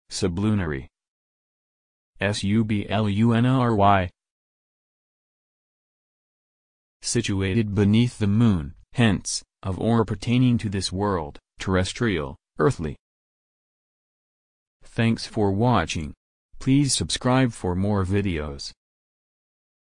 Sublunary Meaning
Video shows what sublunary means. Situated beneath the moon; hence, of or pertaining to this world; terrestrial; earthly. Sublunary Meaning. How to pronounce, definition audio dictionary. How to say sublunary